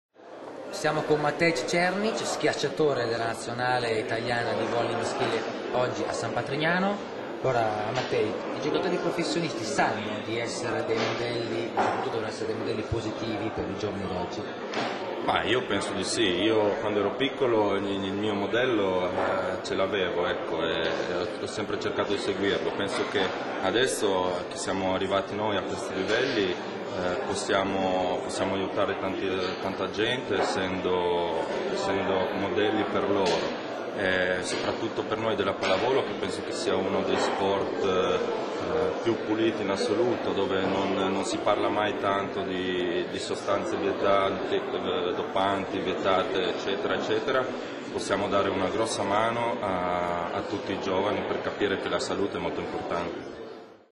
Incontri: Matej Cernic
Matej Cernic della nazionale italiana di volley in visita a San Patrignano